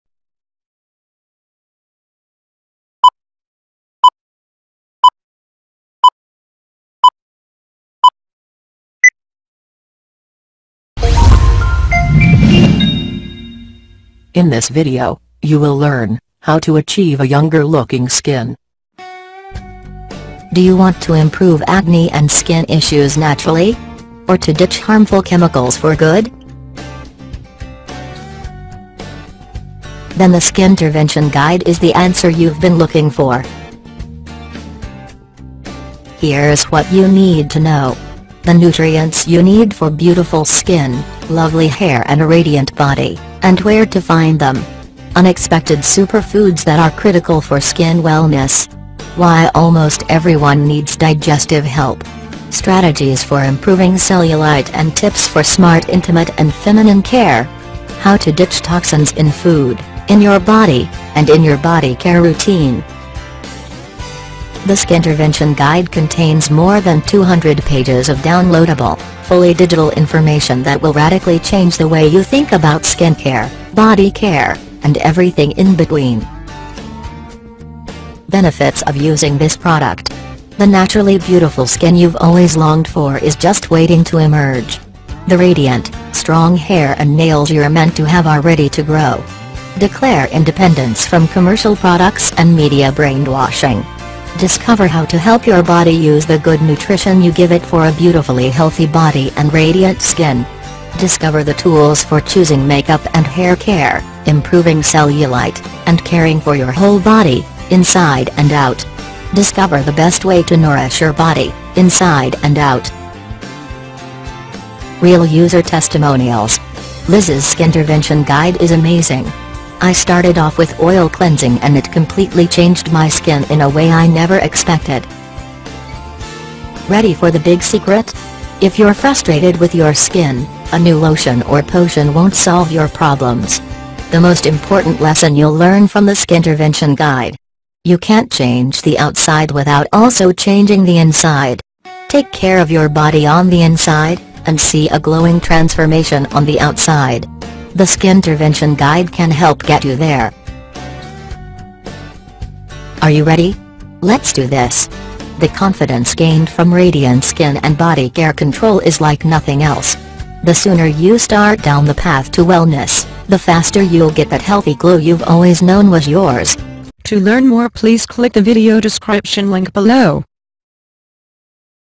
► Skintervention Guide Purely Paleo Skincare
The Purely Primal Skincare Guide has EVERYTHING we’d tackle in a one-on-one consult, WITHOUT the price tag and the long wait. I want to help you RIGHT NOW! The Purely Primal Skincare Guide is the answer you DON’T have to wait for.
I’m overwhelmed by the number of people passionately searching for the secret to healthy, vibrant skin, hair, nails and teeth and the keys to a non-toxic body care routine…and by the number of people who want to ditch unnecessary chemicals and reveal the beautifully healthy skin, hair, nails and teeth they’re born to have. These people are ready to take control – and this guide is ready and waiting to make it happen. Whether you’re wondering what nutrition has to do with acne, eczema, or psoriasis; or which natural, non-toxic products can actually deliver powerful results, the Purely Primal Skincare Guide tackles it all.